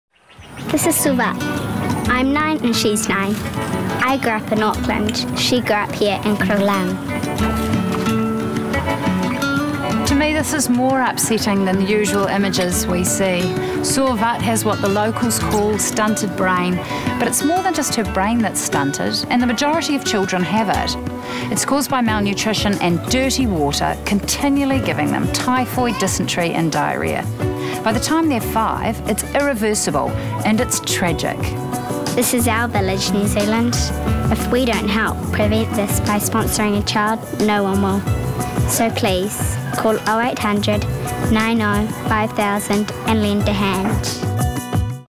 Stunted brain - Lend a hand | Child Sponsorship | 2009
Lend a Hand to a community in need by sponsoring a child with World Vision.